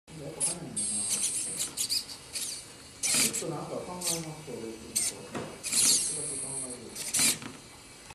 DataGlove&Rockyno2
I made "Data glove". This "Data glove" can control the robot hand of five fingers. (I borrowed the robot hand)
"Rockyno" is a shield board for "Koredeno".

長谷川技術開発
Hasegawa Technology Development